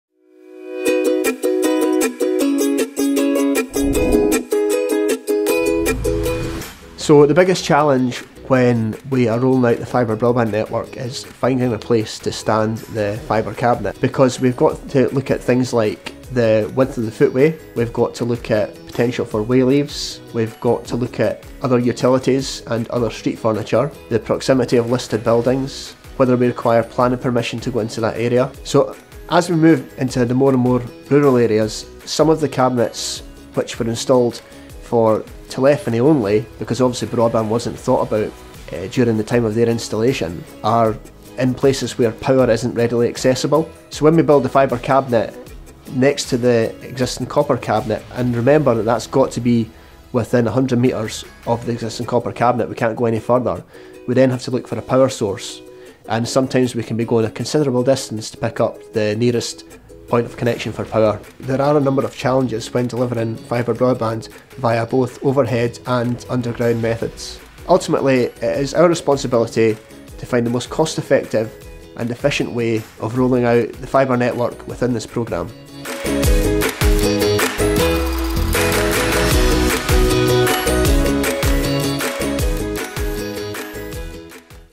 Digital Scotland Superfast Broadband - Challenges1 standing cabinets
Learn about some of the challenges we face in delivering fibre broadband